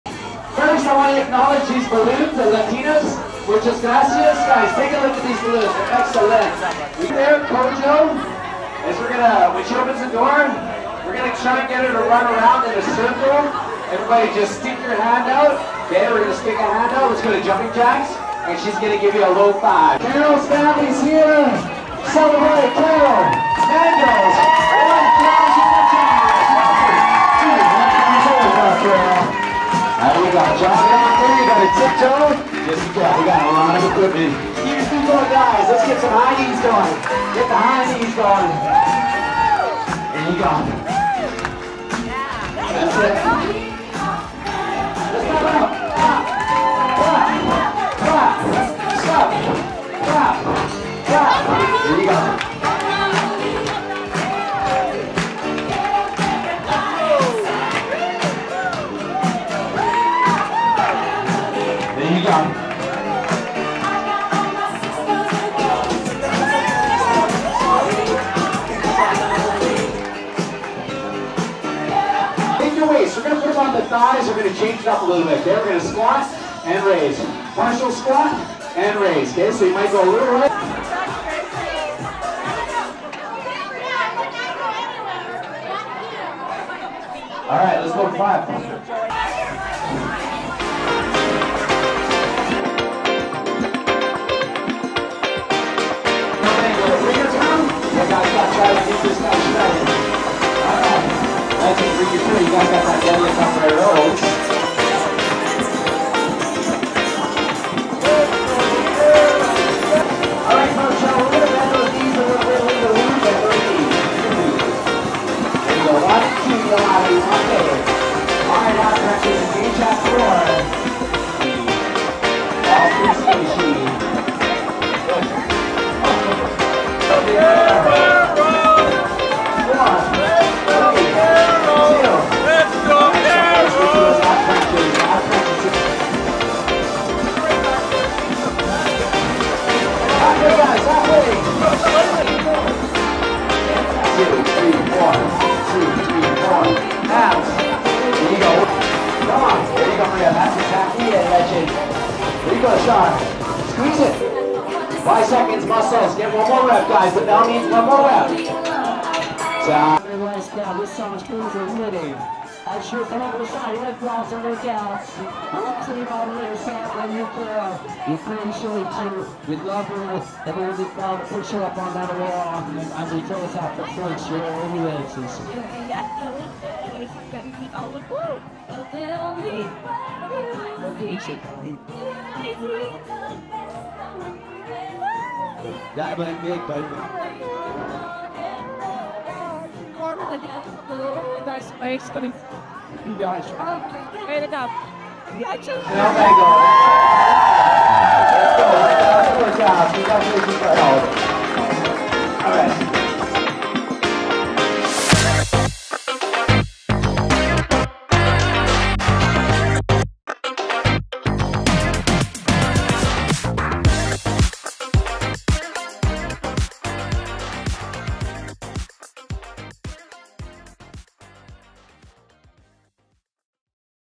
Celebrating Carol’s 1000 Workout Milestone! 🎉
Want to witness something truly inspiring?

In today’s video, I’m celebrating Carol’s incredible 1000 workout milestone at FitClub!

This isn’t just about the numbers—it's a heartfelt tribute to her mother, who taught her the power of true friendship. 💖

Even when life gets tough, Carol’s smile lights up the room, reminding us all that there's always hope.

Let’s cheer her on as she takes her place on the wall of fame!

What does friendship mean to you? 🤔

Challenge: Share your favorite memory with a friend in the comments!

Plus: Try to do one kind thing for someone today! ✨

Where to start with Fit Club?
Kickstart Workout Challenge: 5 days for $5! Text “5-days” to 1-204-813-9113.